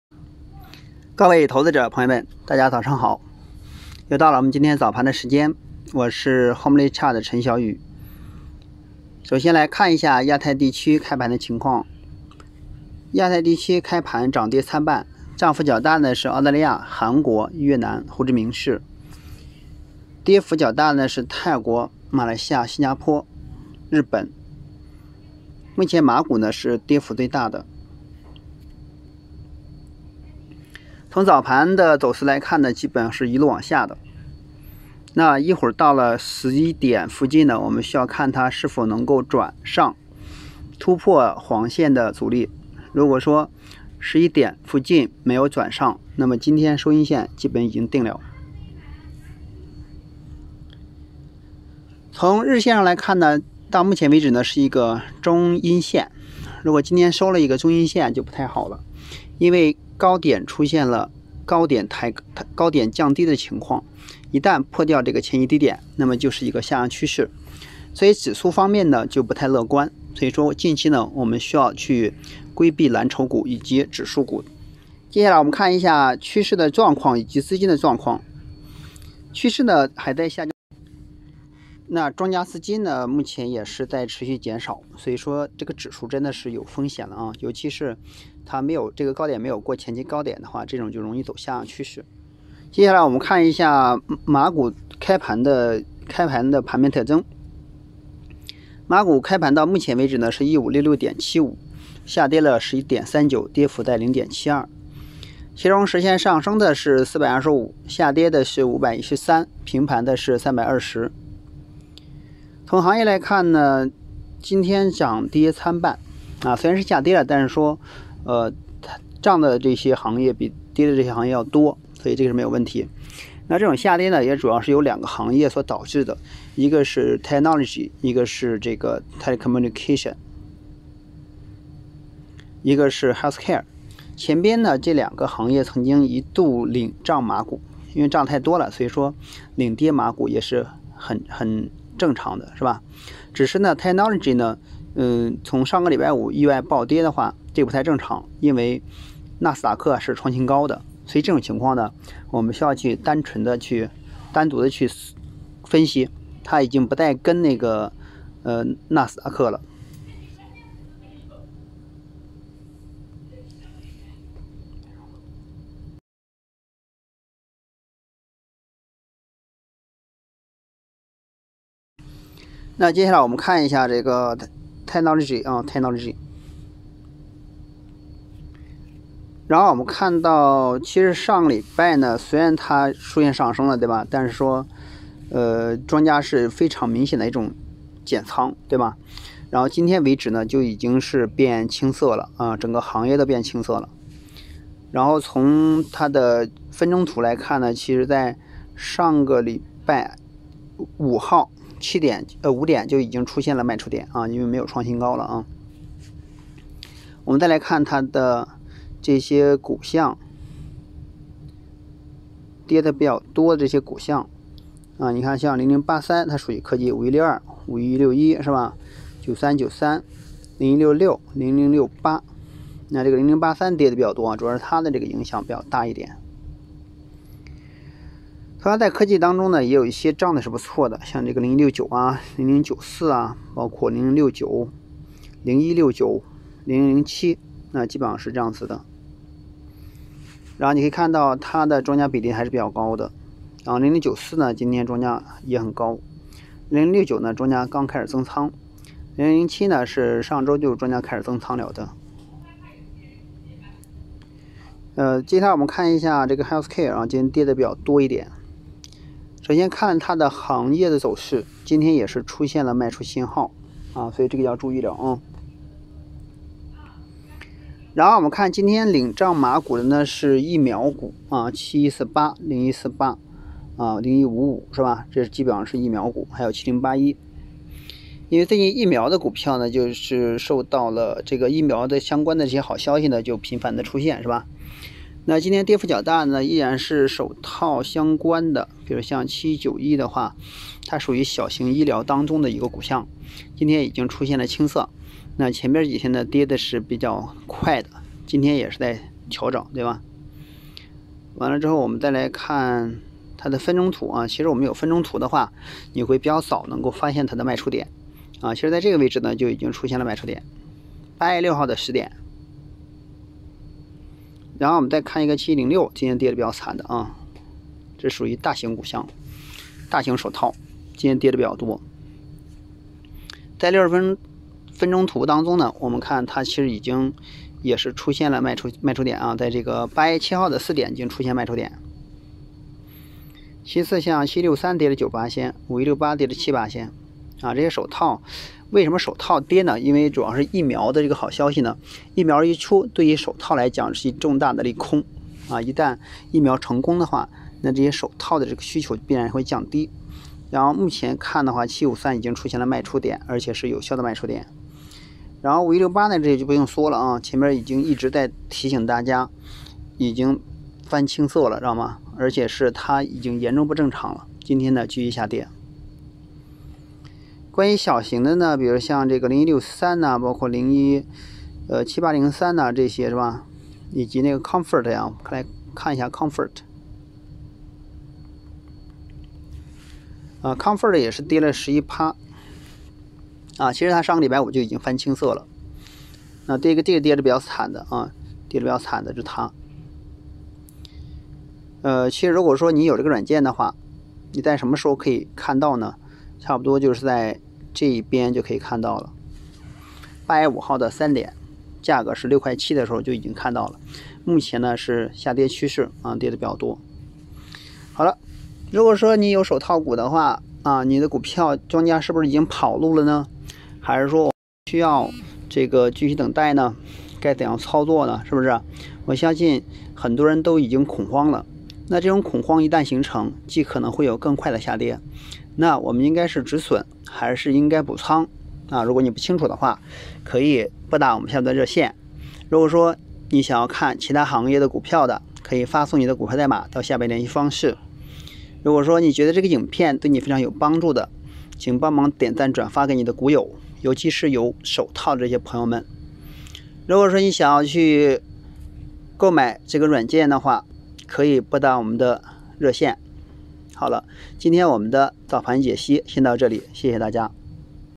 KLCI 为什么持续下跌，科技手套结束上升了吗
① 七上八下不懂就来玩股票能赚吗
② 科技手套再领跌，神话结束？
③ 买高了，手套怎么办？